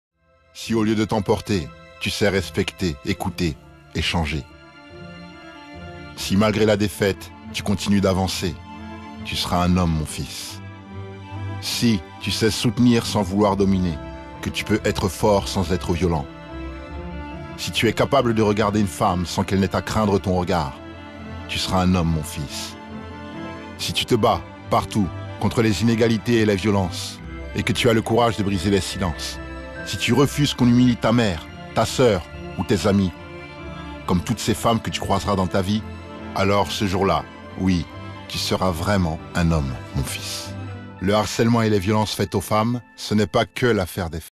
Tu seras un homme mon fils. Etre un homme c'est être fort sans être violent.
Oxmo puccino prête sa voix et sa prose à la fondation des femmes. la violence faite aux femmes repose essentiellement sur l'éducation des garçons.